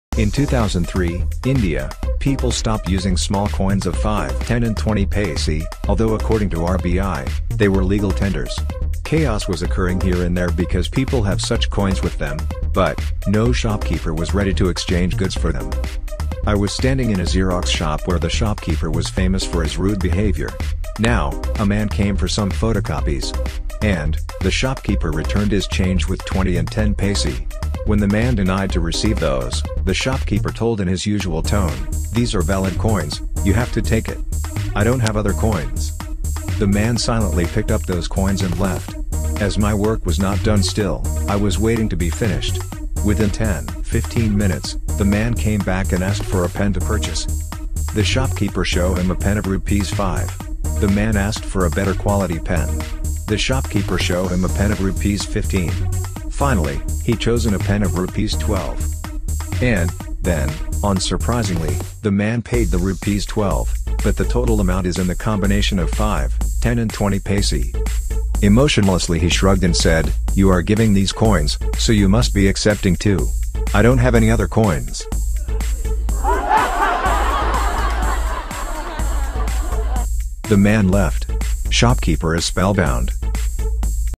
Short Funny Story (Tit for Tat!)
In 2003, India, people stop using small coins of 5, 10 and 20 paise, although according to RBI, they were legal tenders. Chaos was occurring here and there because people have such coins with them, but no shopkeeper was ready to exchange goods for them.

I was standing in a Xerox shop where the shopkeeper was famous for his rude behavior. Now, a man came for some photocopies. And, the shopkeeper returned his change with 20 and 10 paise. When the man denied to receive those, the shopkeeper told in his usual tone: "These are valid coins, you have to take it! I don't have other coins!!"

The man silently picked up those coins and left. As my work was not done still, I was waiting to be finished. Within 10, 15 minutes, the man came back and asked for a pen to purchase.

The shopkeeper show him a pen of Rupees 5. The man asked for a better-quality pen. The shopkeeper show him a pen of Rupees 15. Finally, he chosen a pen of Rupees 12.

And, then, on surprisingly, the man paid the Rupees 12, but the total amount is in the combination of 5, 10 and 20 paise.

Emotionlessly he shrugged and said: "you are giving these coins, so you must be accepting too! I don't have any other coins!!"

The man left. Shopkeeper is spellbound!!